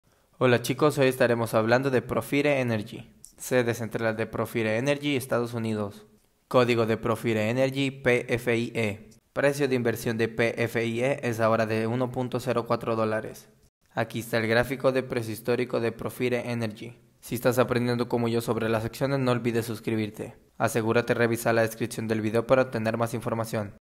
🔴 Datos Sobre El Comercio De Acciones De Profire Energy Inc PFIE 🔴
Profire Energy es una empresa de tecnología de yacimientos petrolíferos que se especializa en el diseño de sistemas de gestión de quemadores y otras tecnologías de gestión de la combustión.